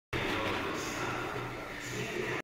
37a-stretching base-basic:bassa schiena, glutei-lower back, gluteus
la sera prima di coricarsi,  o alla fine dell'allenamento fare un defaticamento (cool down) composto da: 2 minuti di camminata lenta con profonde respirazioni fare 10-15 minuti di stretching statico passivo . Mantenere ogni posizione mostrata per 10-60 secondi su ambo i lati del corpo. Questi esercizi possono essere anche fatti a scopo terapeutico per problemi osteoarticolari di vario genere.